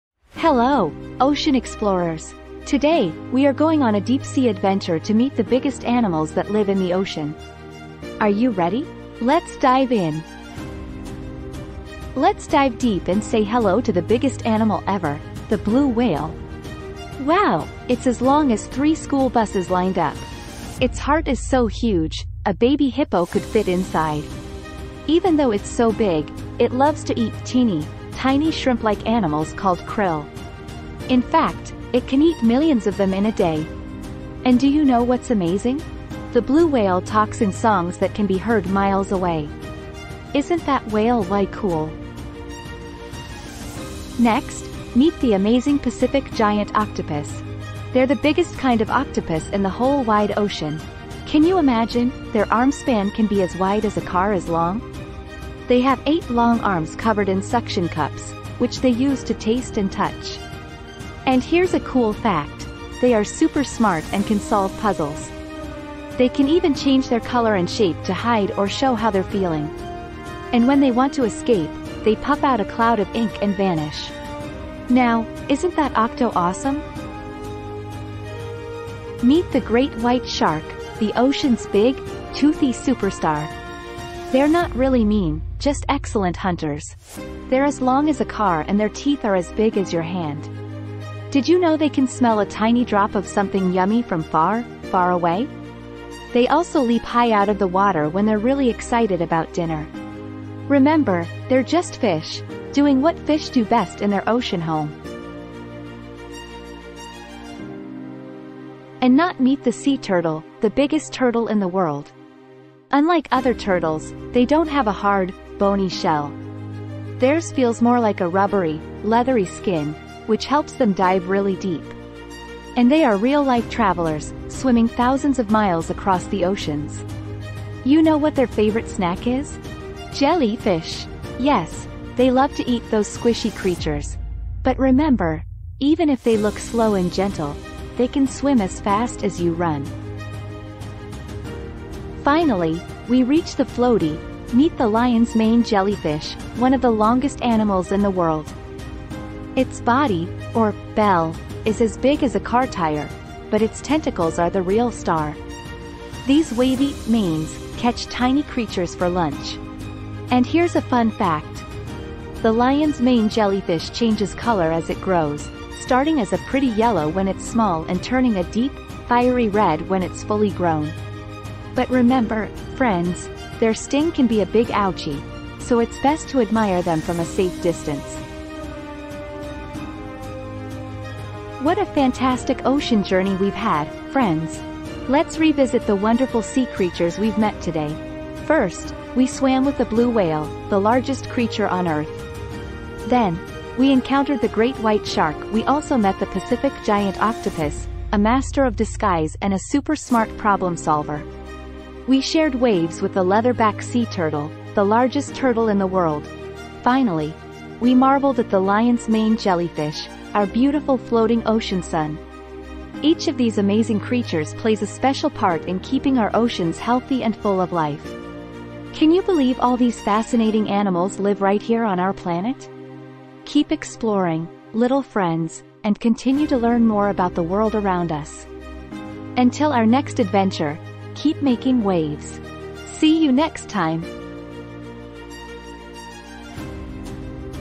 Ocean Giants: Top 5 Largest Sea Creatures for Kids!
"Dive into an amazing underwater journey with us as we explore the Top 5 largest sea creatures in the world! In this fun, educational video for kids aged 4-6, we meet:

The Blue Whale: The biggest
The Pacific Giant Octopus: A master of disguise with eight long arms and super problem-solving skills!
The Great White Shark: The ocean's toothy superstar with a super sense of smell!
The Leatherback Sea Turtle: The world's largest turtle who loves long swims and jellyfish snacks!
The Lion's Mane Jellyfish: Our beautiful, floating ocean sun with 'manes' as long as a blue whale!
Remember, our oceans are home to some of the most amazing creatures. Join us on this underwater adventure and discover just how incredible our blue planet is. OceanGiants KidsEducation SeaCreatures"